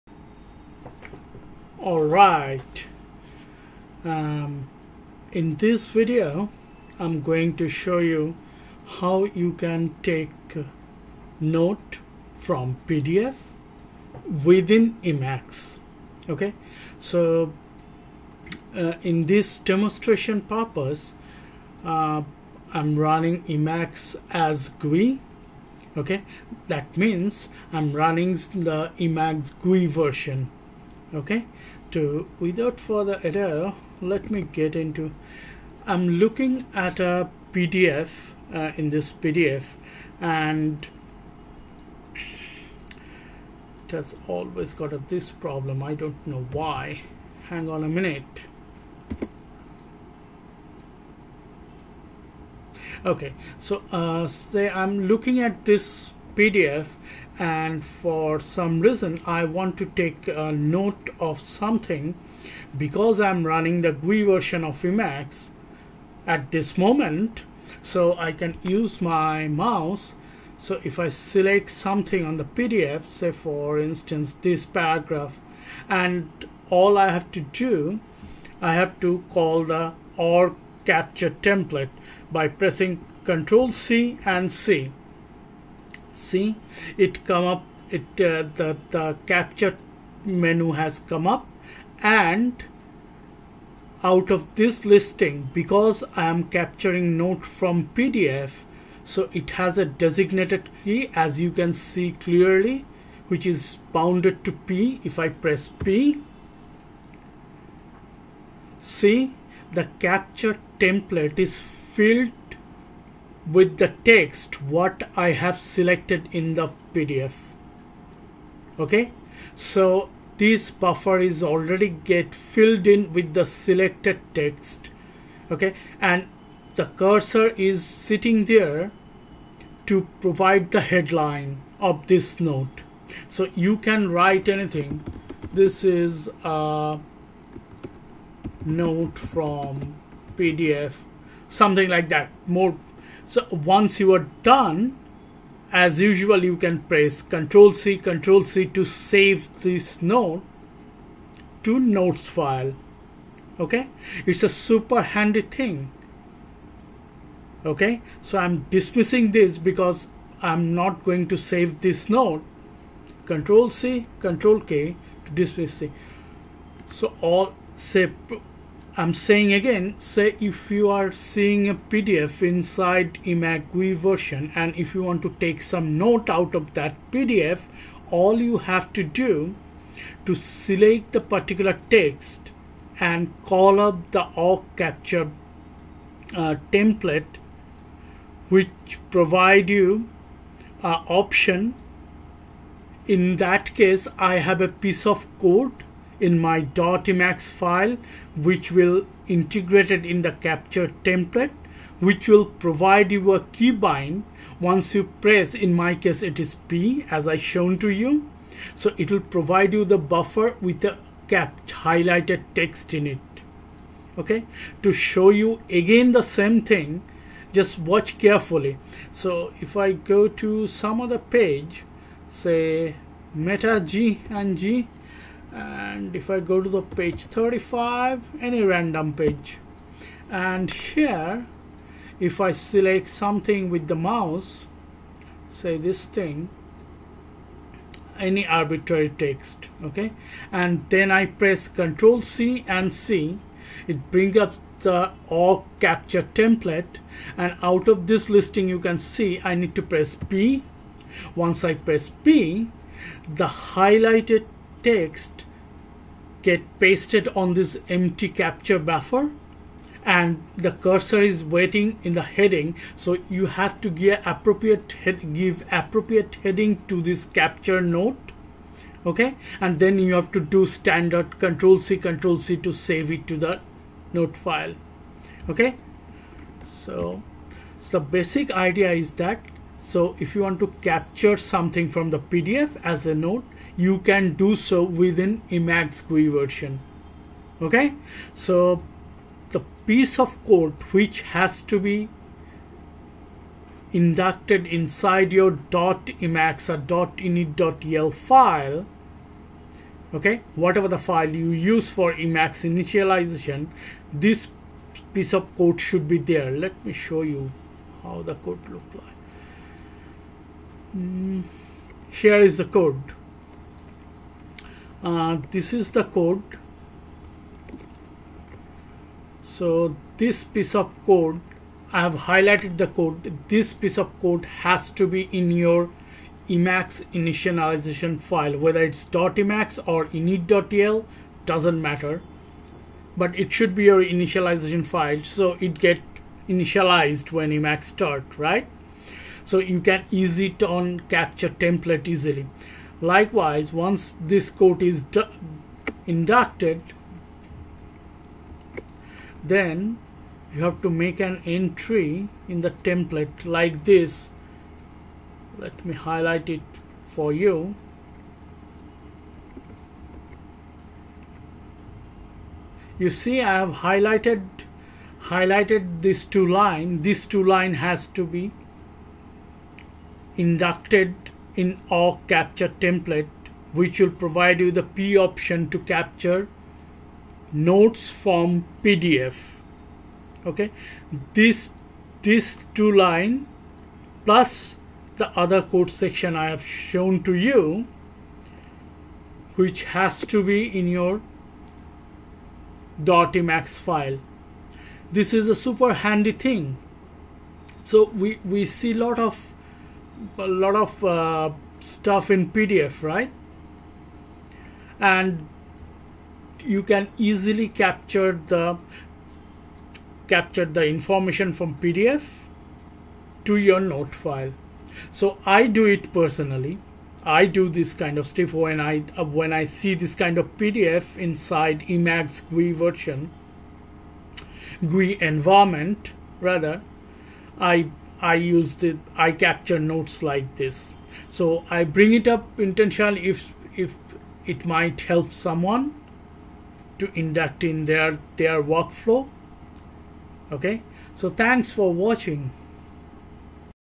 Emacs Notes Capture From PDF 2022 05_01_18:35:00
In this video, you will see, how easily you can take note from a pdf and store it in an org notes file.